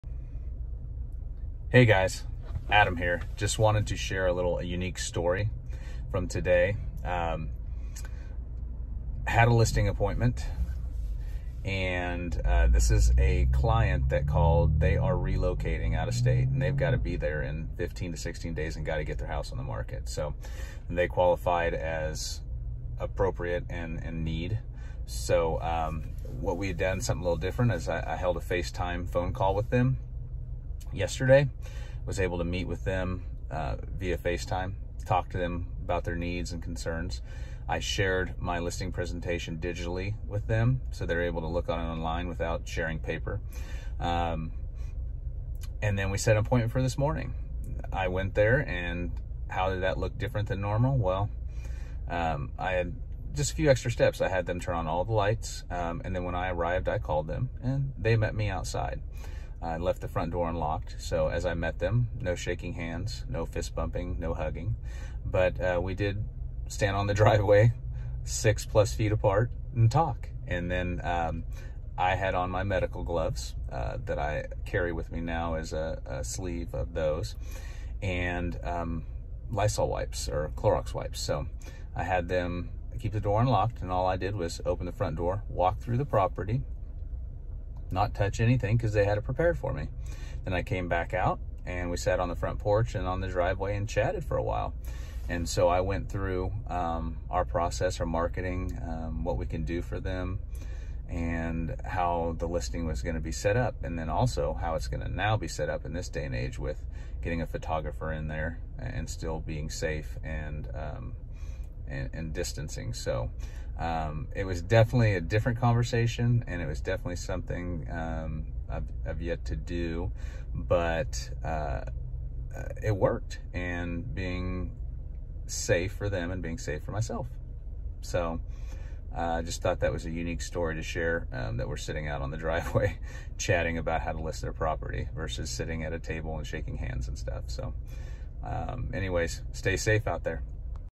How to handle a must list in this climate
Social distance and safety for those who must sell now